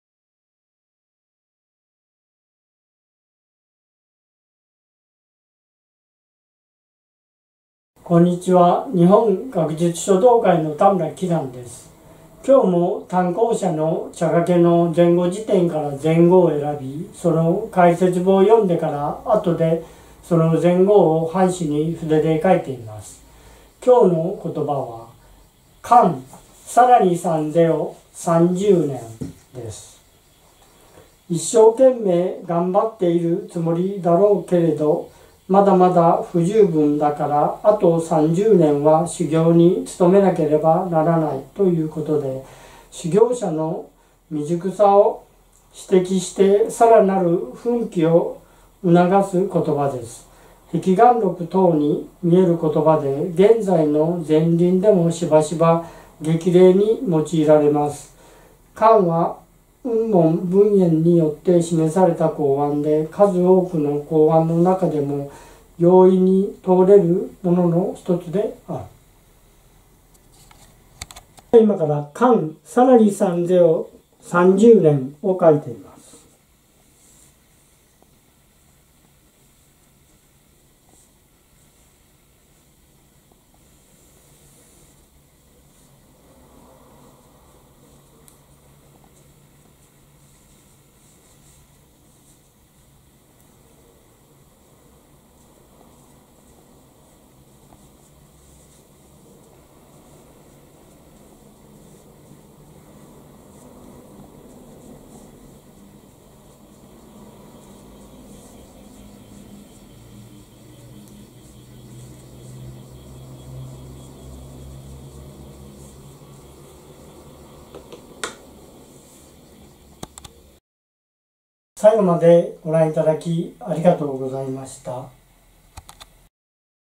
田村季山先生による禅語解説と範書「関 更参三十年」
本日は「関　更参三十年（かん　さらにさんぜよさんじゅうねん）」です。